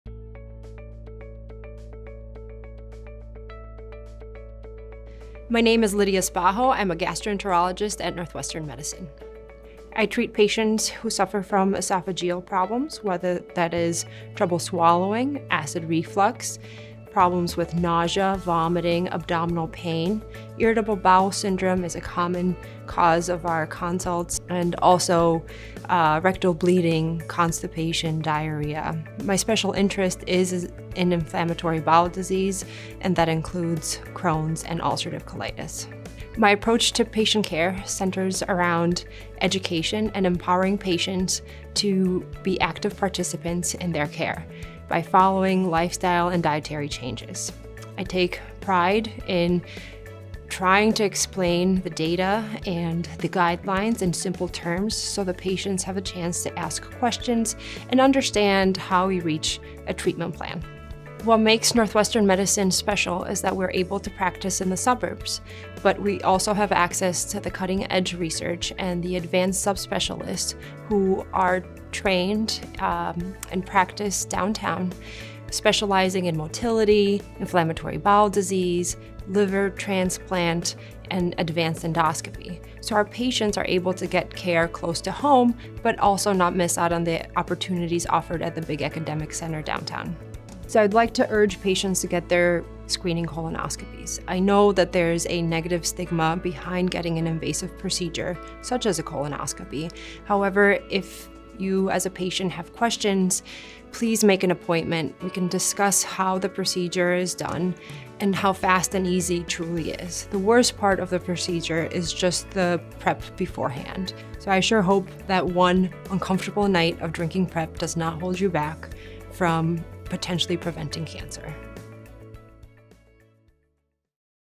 Lidia Spaho, MD
Lidia Spaho, MD is a Gastroenterology physician at Northwestern Medicine in the western suburbs.